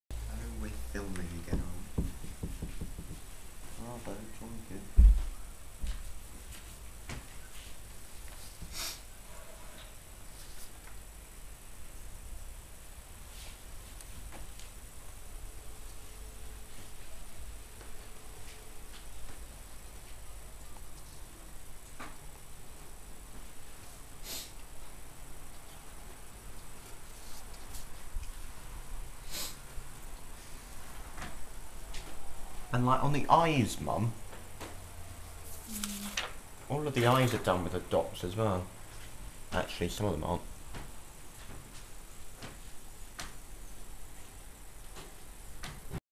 Alex does Lichtenstein
Alex paints himself in the style of Roy Lichtenstein. 12-4-09